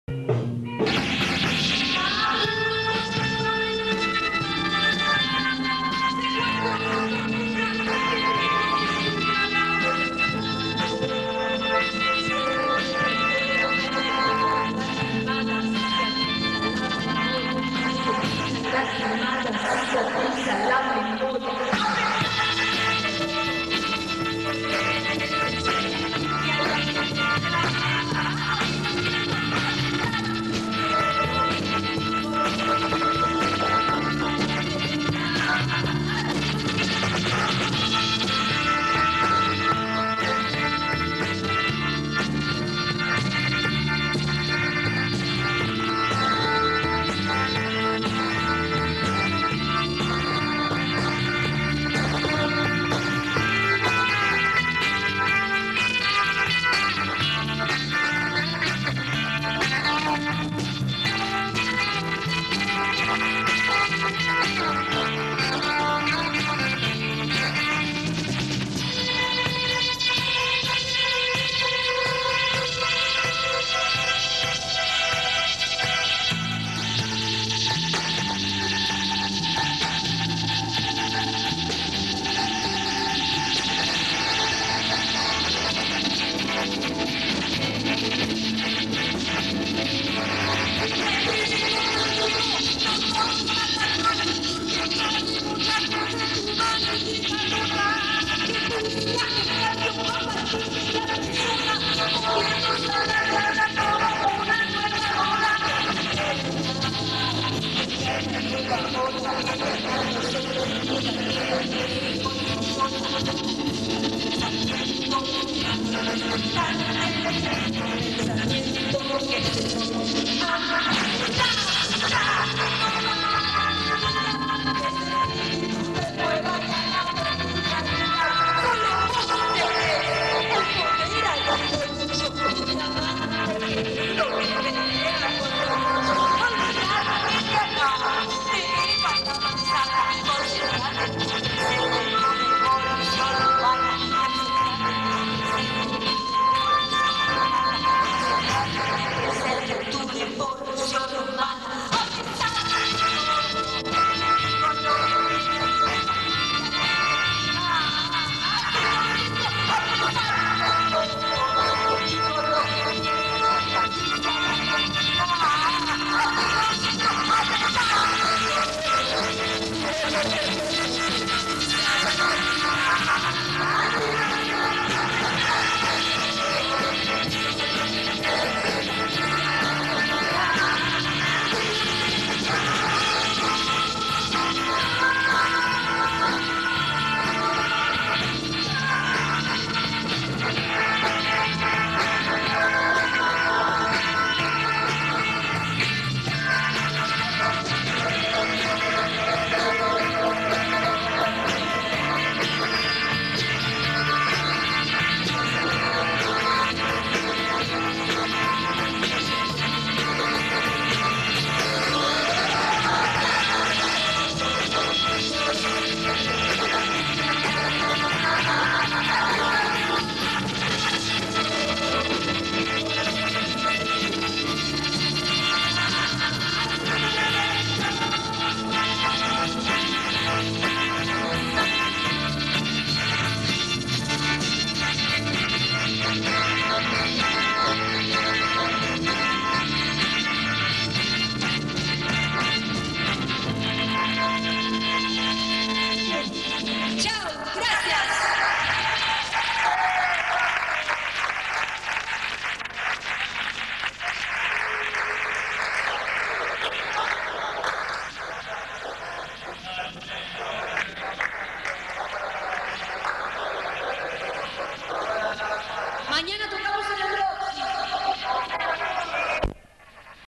Patricias Argentinas ( Karina K & Grupo Rock) / video hugoMARviggiano
Grupo de Rock Argentino integrado por Mujeres, entre ellas, la actriz Karina K
Filmacion Hugo Omar Viggiano.siglo XX